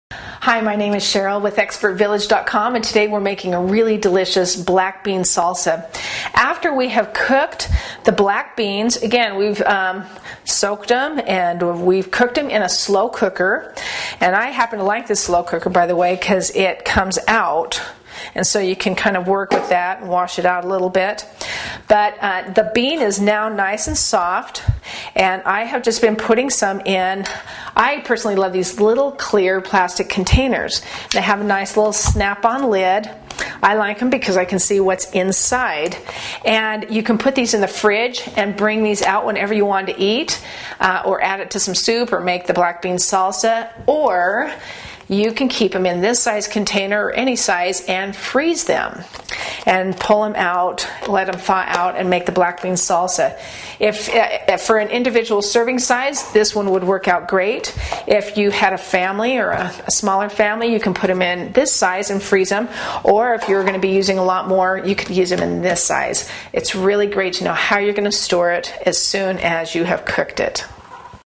How to Store Black Beans
How to Store Black Beans. Part of the series: How to Make a Black Bean Salsa. Once beans are cooked, tips on repackaging them for storage in the refrigerator in clear containers; learn this and more in this free online cooking video taught by an expert chef.